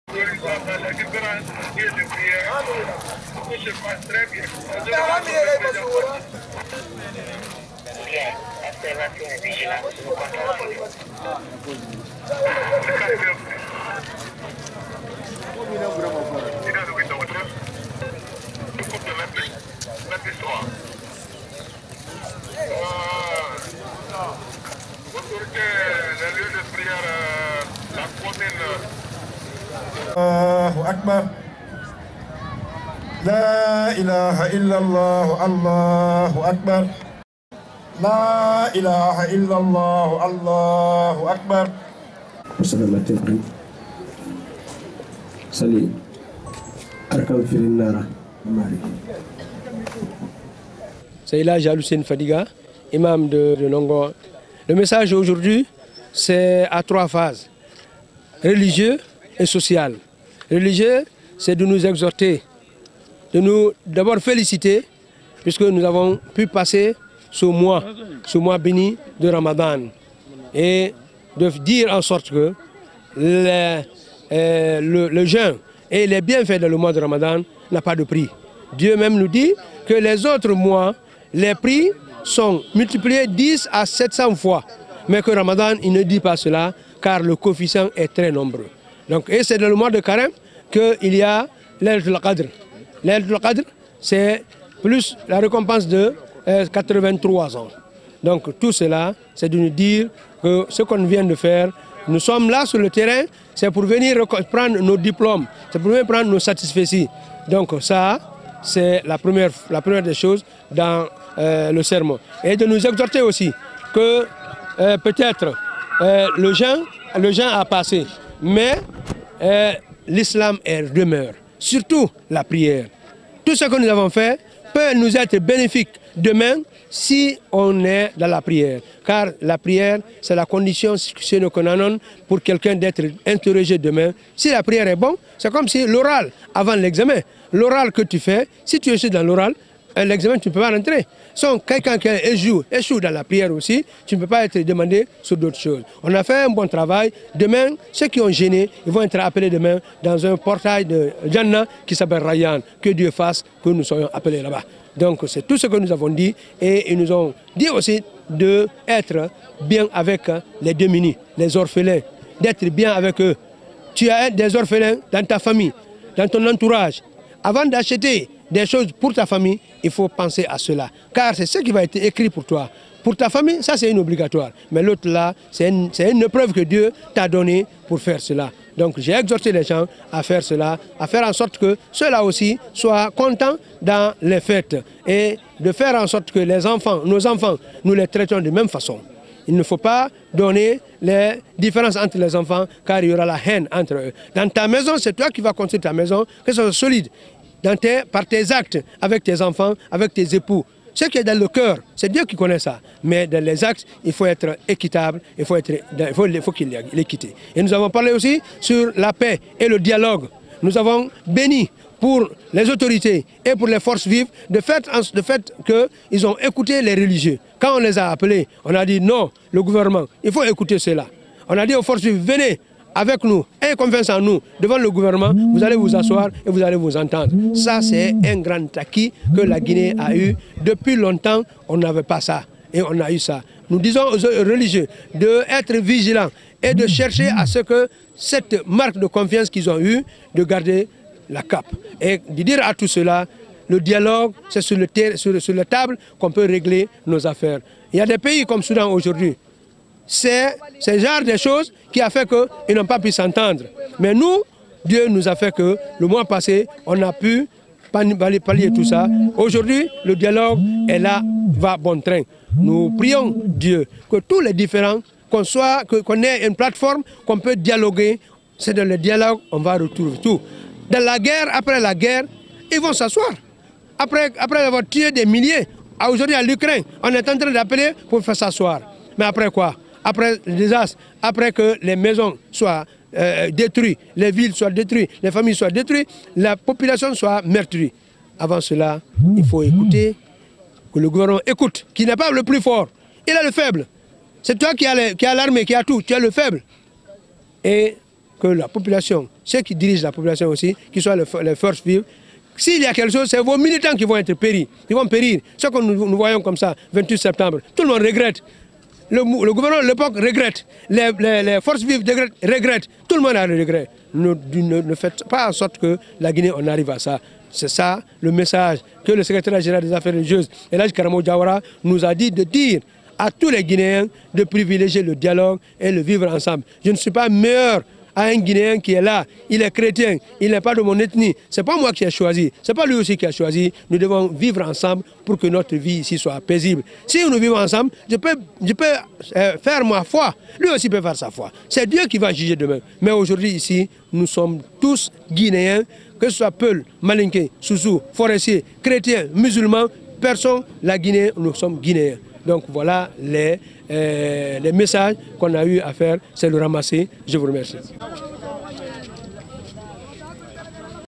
Fête du Ramadan et Transition en Guinée : Elhadj Hussein Fadiga parle du dialogue politique
Les fidèles musulmans ont célébré ce vendredi, 21 avril 2023, jour qui marque la fin du mois de Ramadan. Dans l'enceinte du stade de Nongo, c'est l'imam de la Mosquée de Nongo Taady qui a dirigé la prière. Dans son sermon, l'imam a rappelé les bienfaits du Ramadan, il a ensuite exhorté les musulmans à être aux côtés des démunis et fin, il a invité les religieux à être vigilants par rapport au dialogue en cours entre les le gouvernement et les forces vives de Guinée